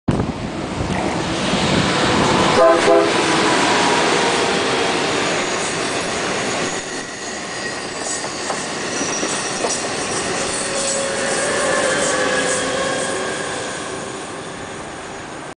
Acela at Nan interlocking going westbound
Me and W12CO1 went out train watching and our first train is this westbound acela express with some nice horn here